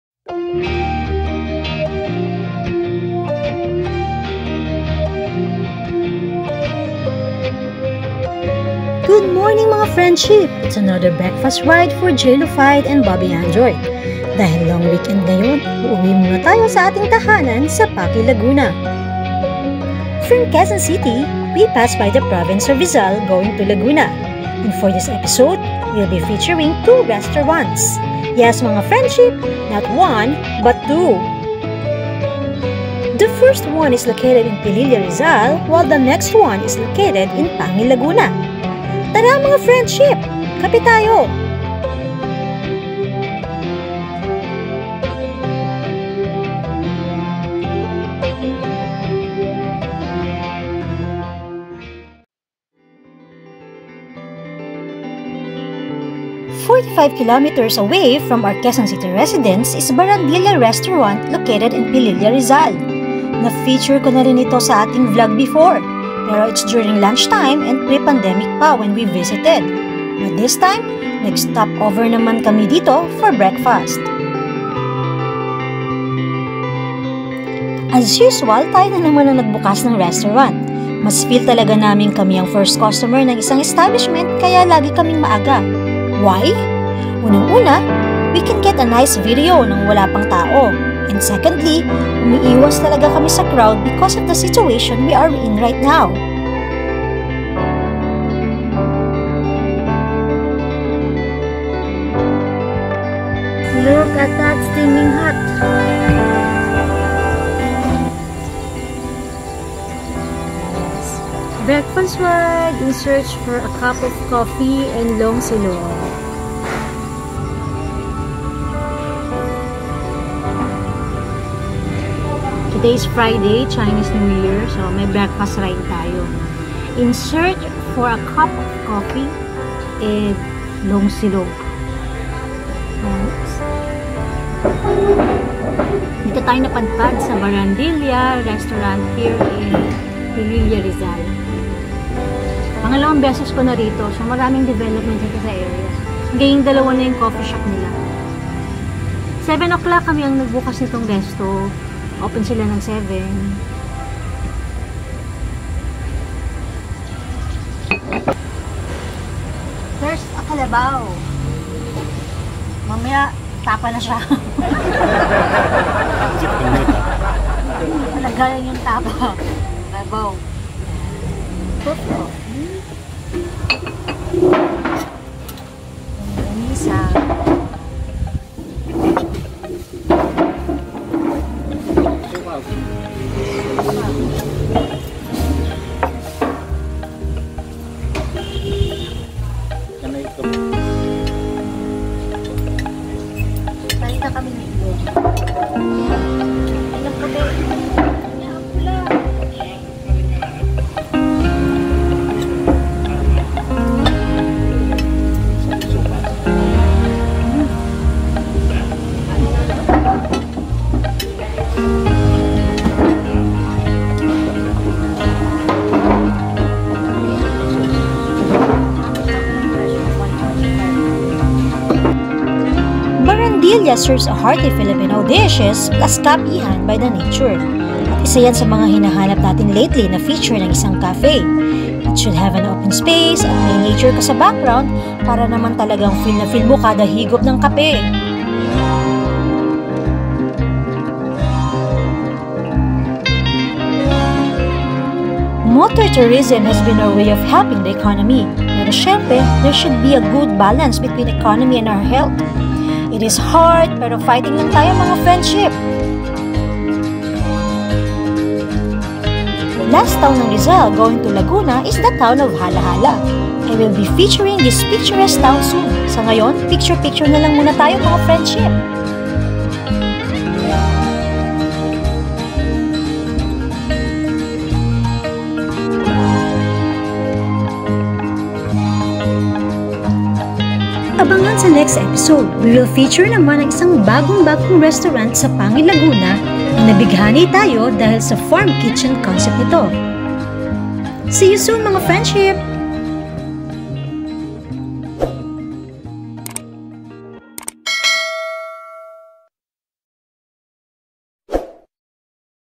NAGKAPE KAMI KASAMA ANG KALABAW | BREAKFAST RIDE MANILA - RIZAL - LAGUNA | BARANDILYA KAPIHAN
Part 1 episode of our Breakfast ride during the long weekend. From Quezon City to Rizal to Laguna riding our NMax motorcycle named Emax.

Featuring Barandilya Restaurant and Event Hall + Kapihan